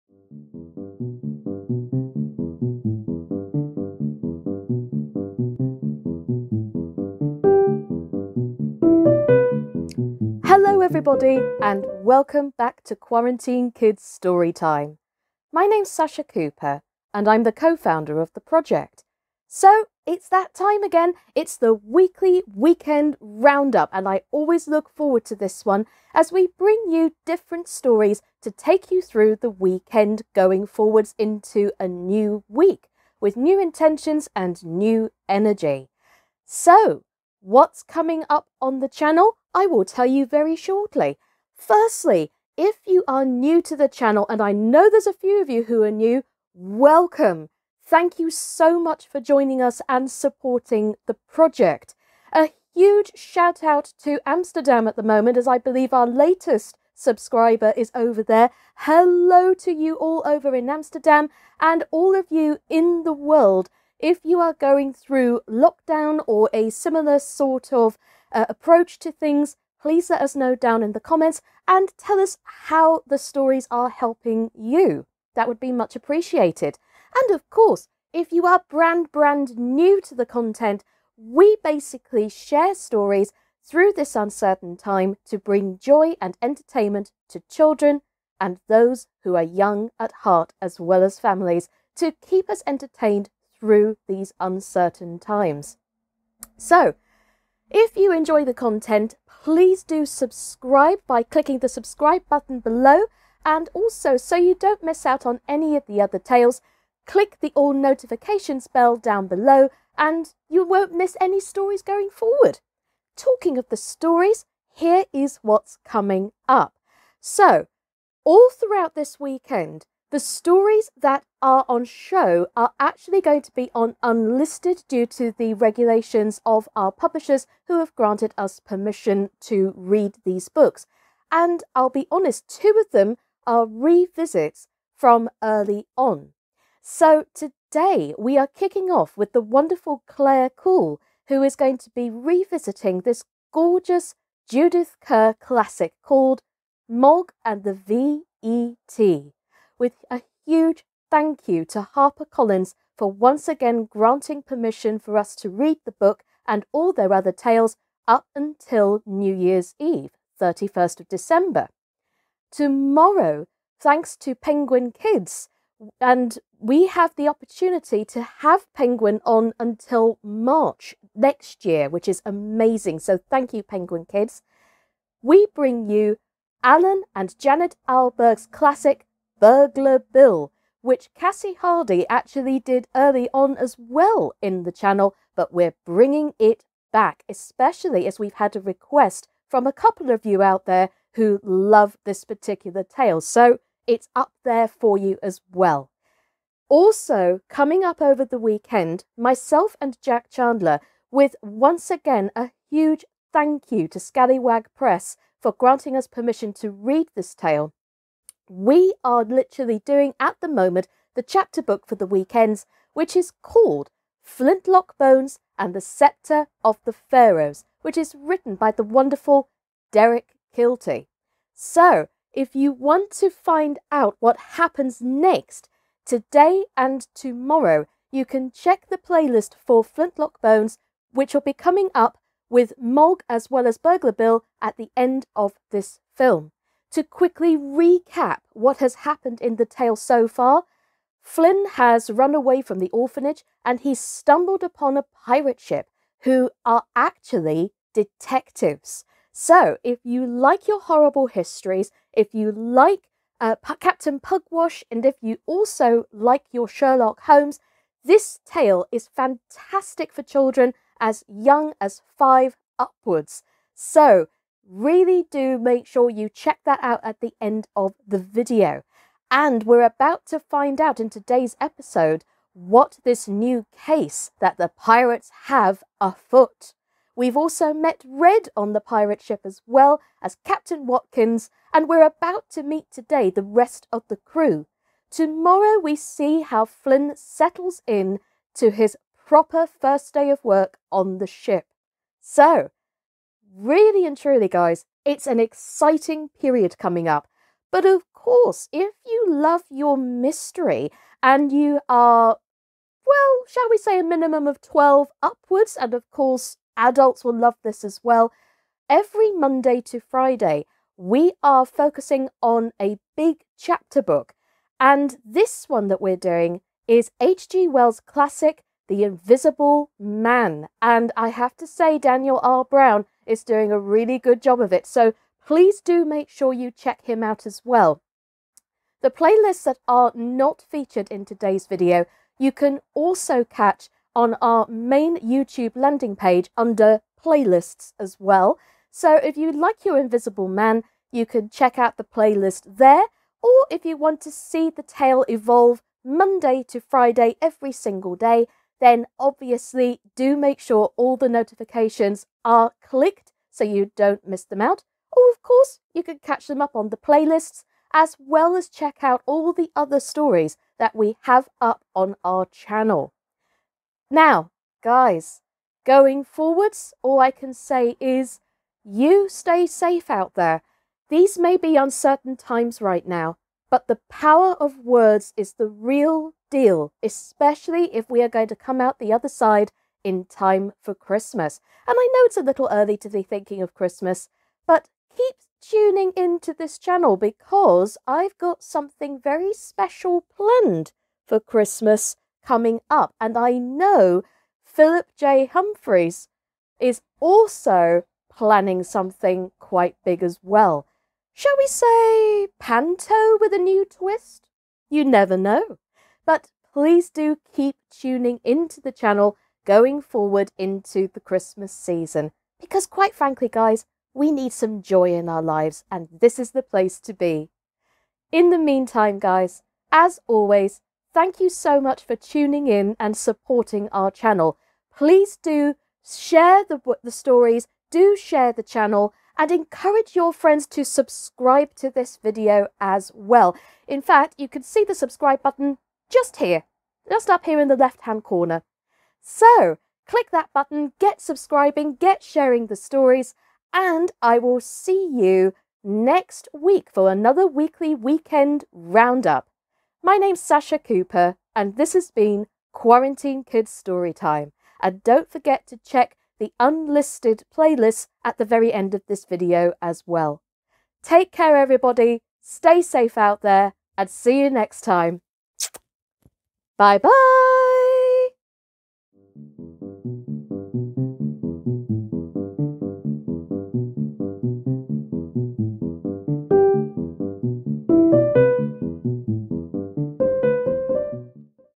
Weekly Weekend Round Up 7th November 2020 With Sascha Cooper
Welcome to Quarantine Kids Storytime :) These are the next lot of weekend stories

Claire Coull revisits Mog and the Vee Ee Tee today, Written and Illustrated by Judith Kerr. A huge thank you to Harper Collins Childrens Books for granting permission with their license till 31st December 2020.

Kassie Hardy revisits Burglar Bill tomorrow (Sunday), written by Alan and Janet Ahlberg. A huge thank you to Penguin Kids for granting permission with their license till 31st March 2021.

Sascha Cooper and Jack Chandler continue Flintlock Bones and the Sceptre of the Pharaohs, by Derick Keilty, Illustrated by Mark Evans today and tomorrow (With special thanks to Scallywag Press LTD for granting permission and granting their license till 31st December) You can buy the book here if you enjoy it

The Invisible Man, By HG Wells, Read By Daniel R Brown continues next week

We also have a special project planned for Halloween...The Raven by Edgar Allen Poe